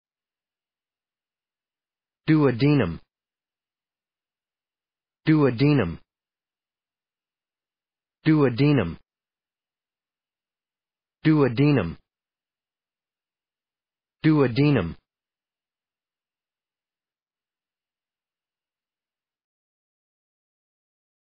How to pronounce duodenum
duodenum American English pronunciation. How to pronounce duodenum correctly. How to say duodenum in proper American English.